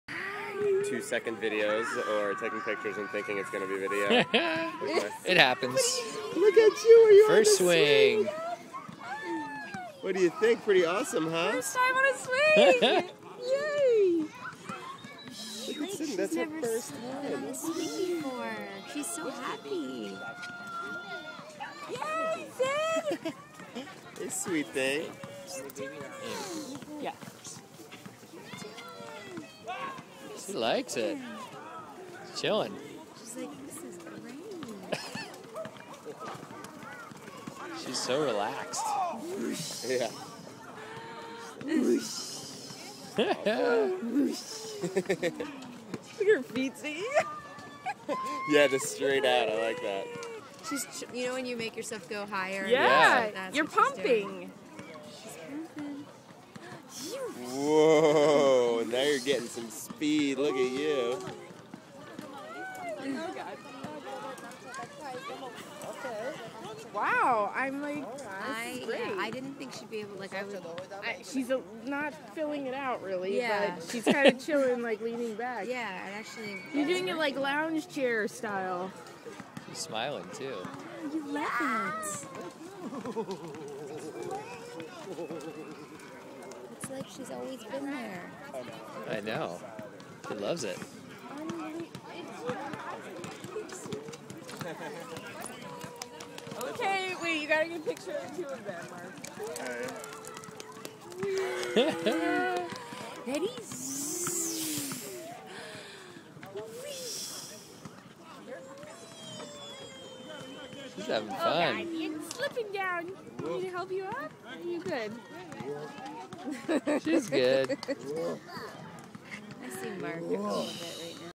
Syd's First Swing
Sydney's first ride on the swing!  5 mos, 3 wks old